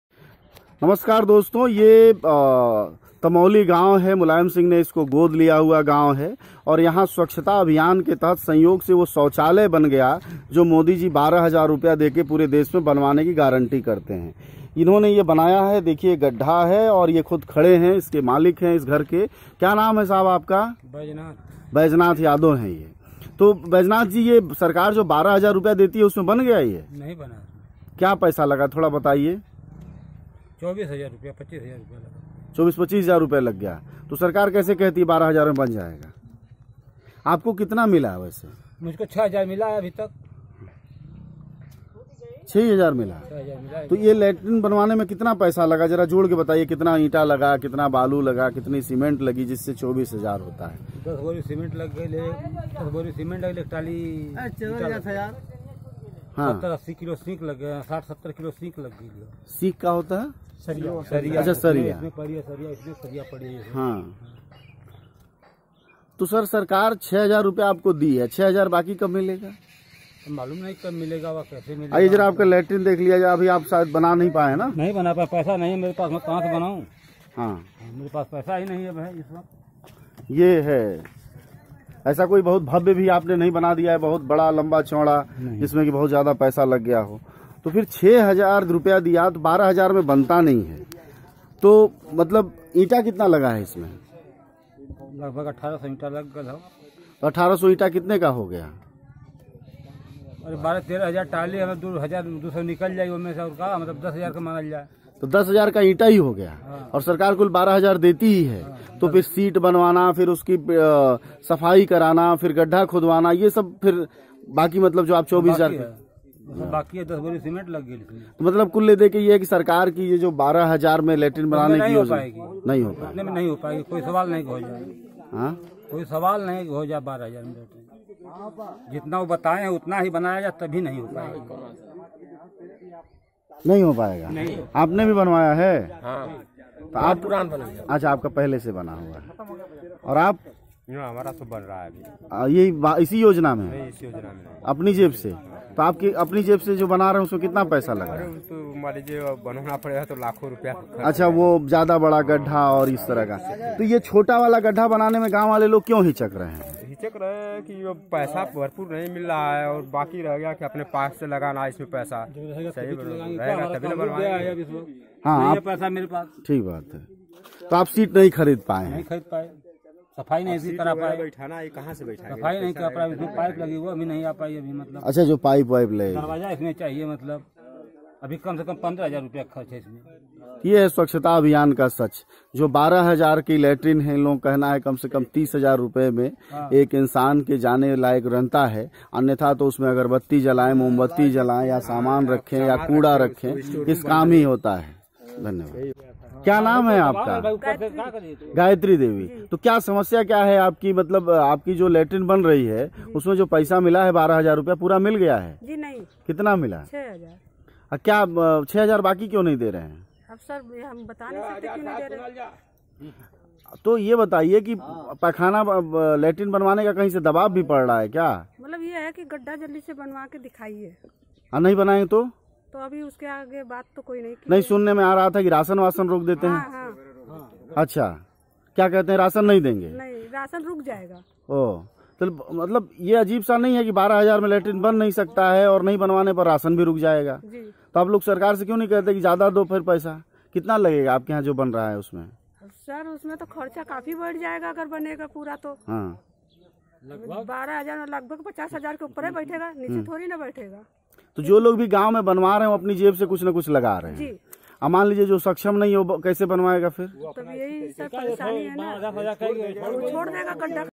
12 हजार में सरकारी शौचालय बनाने की हकीकत है कुछ और
कैसे मोदी सरकार के स्वच्छता अभियान के तहत बनने वाले शौचालय आम गरीबों के गले की आफत बन गए हैं और शौचालय नहीं बनवाने पर सरकार रोक रही गरीबों का राशन